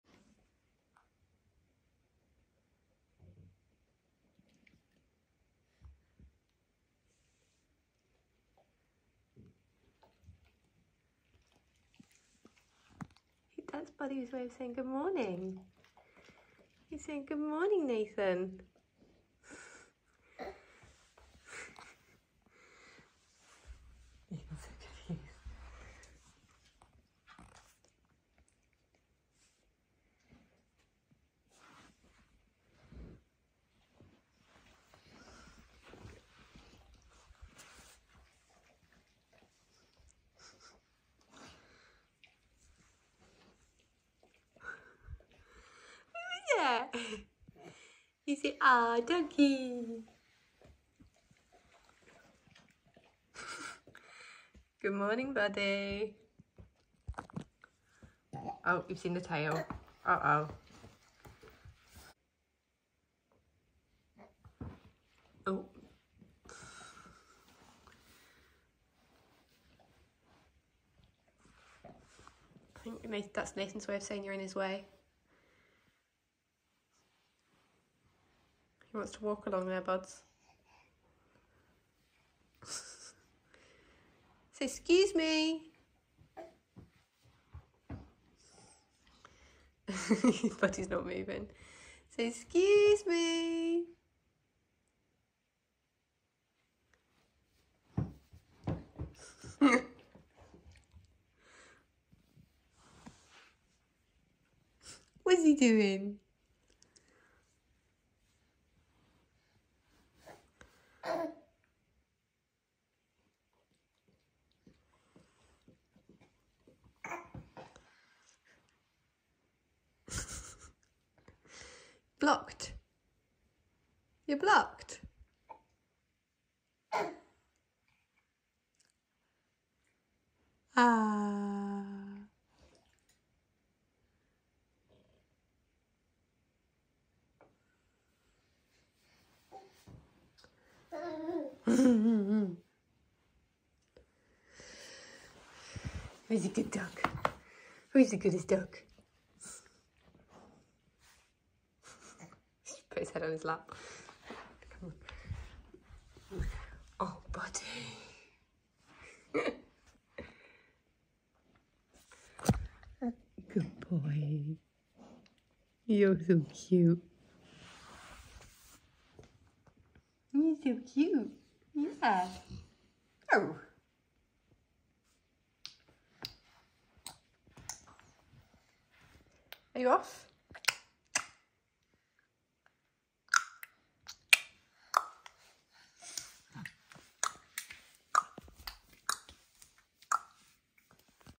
Adorable Baby Boy Loves His Golden Retriever Pup! (Cutest Ever!!)
Buddy doesn't get to see Nathan that much and when he does he's grown each time, he loves him though and follows him round like a shadow. Nathan gets confused by all the dogs at this house haha but he sure does like Buddy. If you eave them to it Nathan just starts to sit in front of him and does his baby shouting at him super cute when Buddy does the confused head tilting.